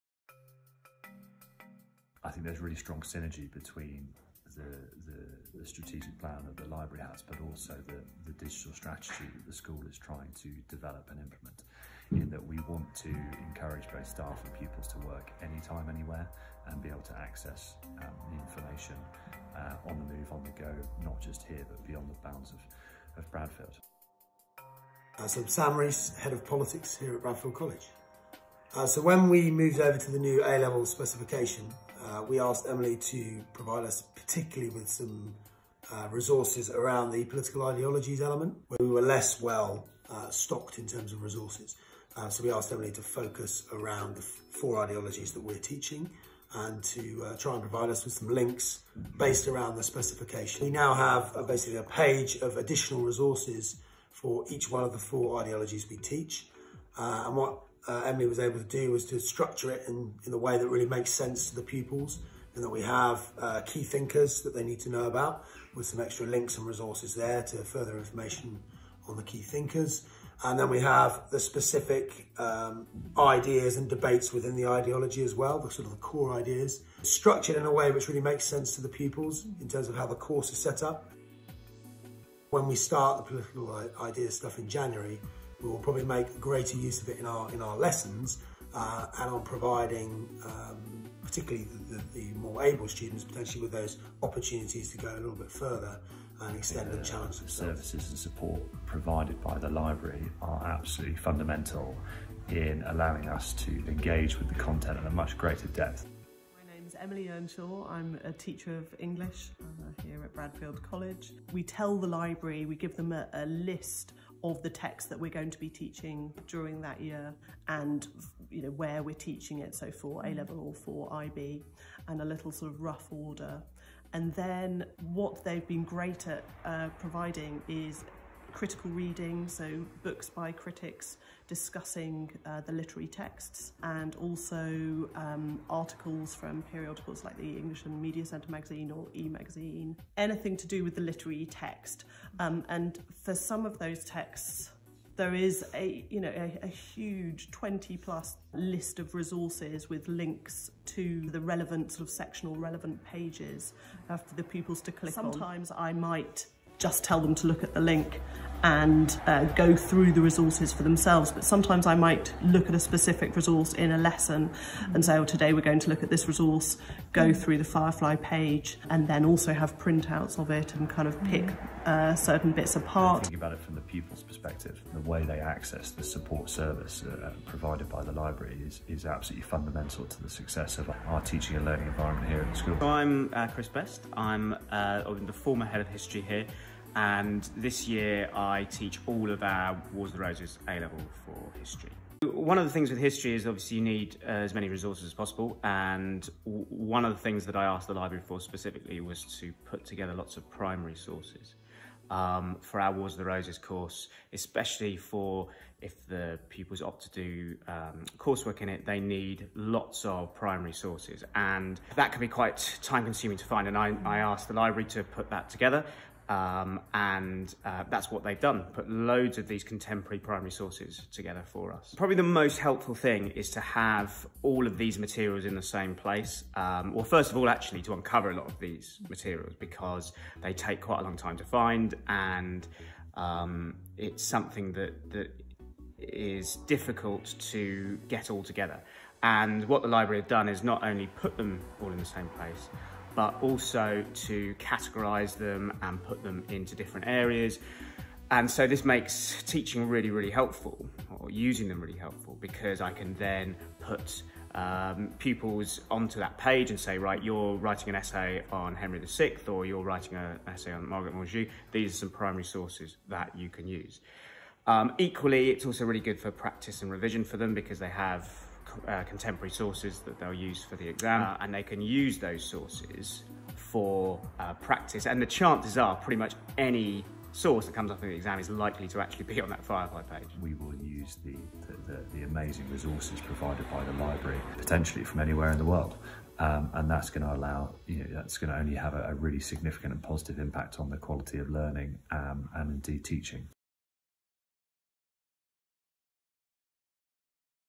Emily Stannard - Librarian - Bradfield College - Interviews
Here Emily Stannard, librarian from Bradfield College interviews teachers from three departments (English, Politics and History) that she has been closely working with over the past couple of terms to see how the work of the library has made a material impact on their teaching.